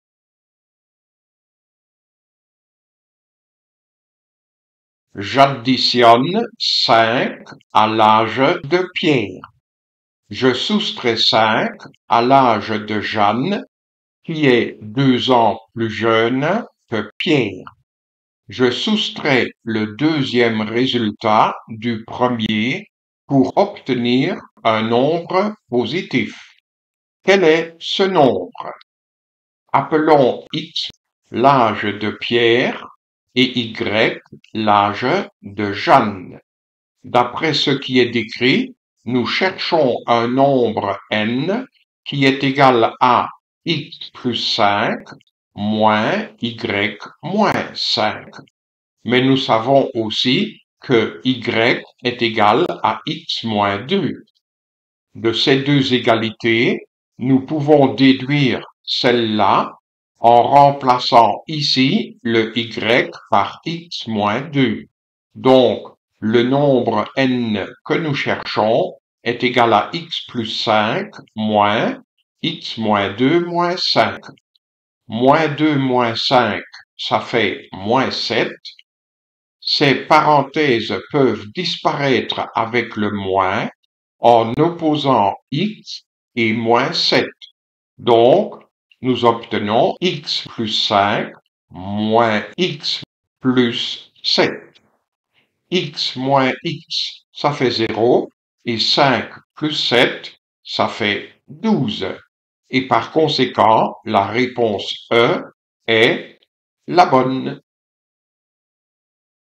Olympiade Mathématique 2024 mini 072
Nous résolvons une tâche de l'olympiade mathématique belge 2024 mini